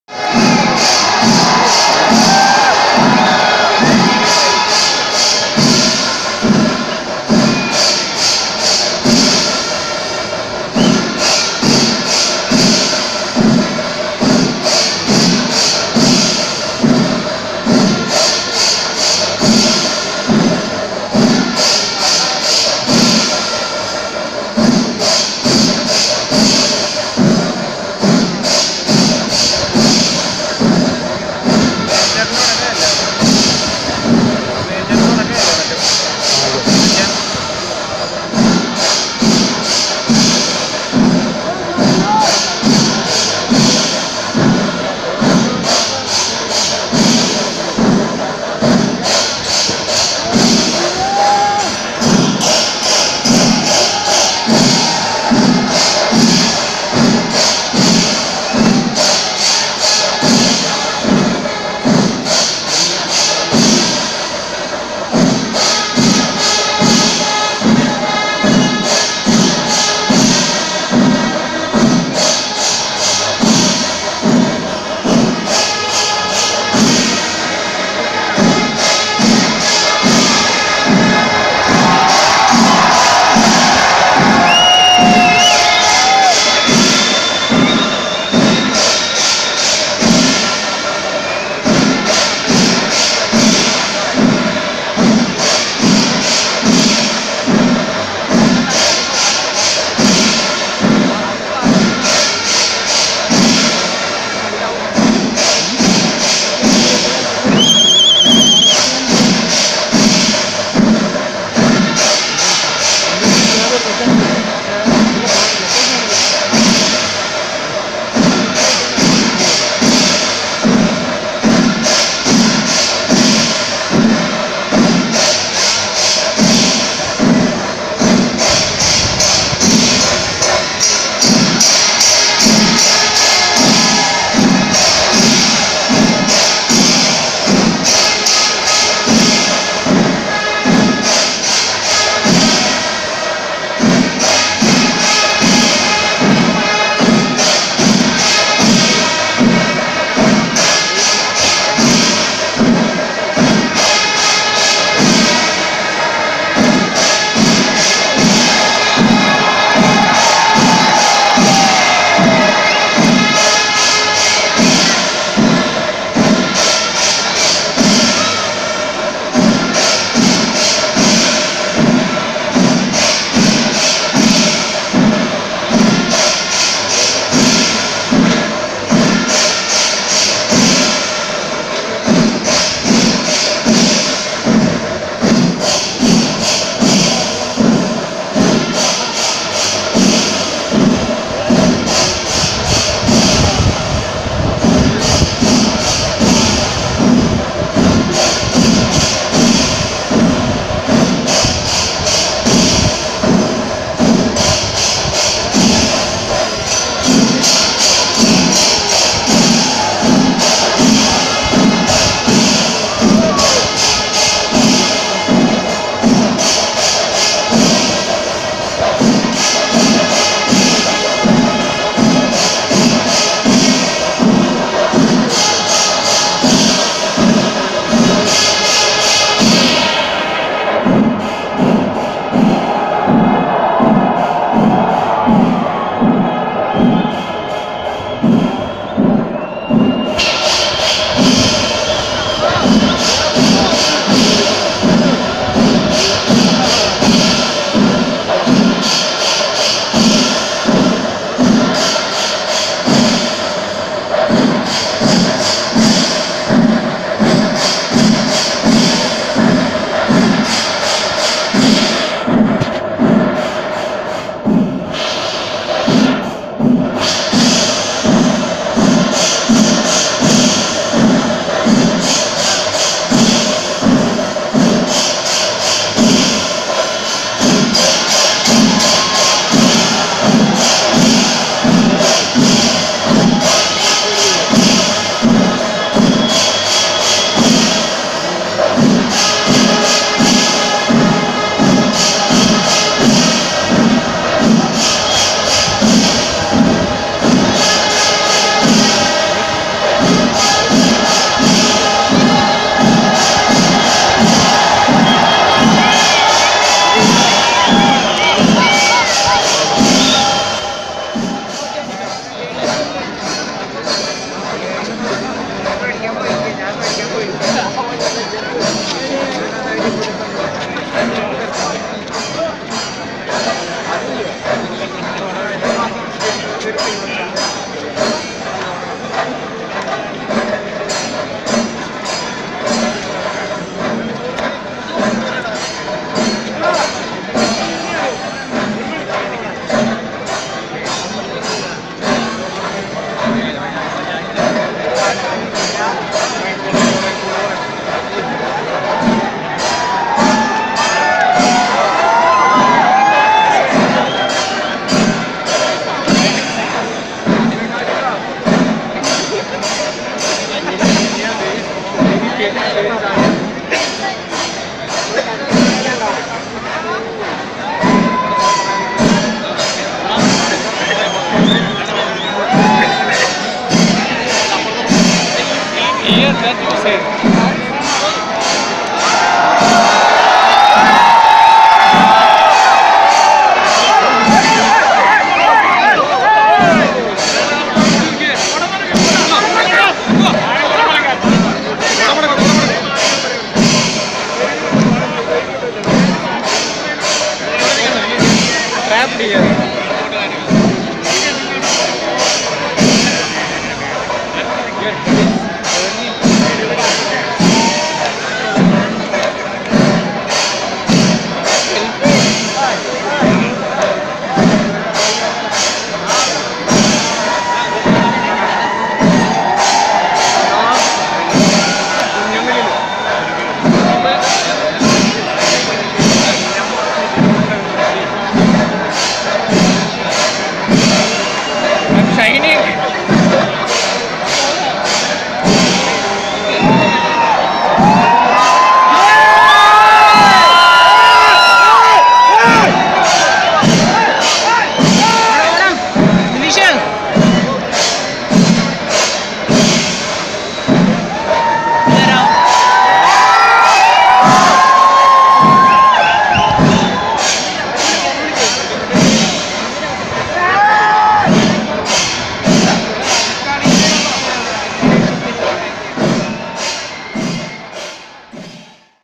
Thrissur pooram 2018 തൃശ്ശൂർ പൂരം
Thrissur Pooram is one of the largest temple festivals in Kerala.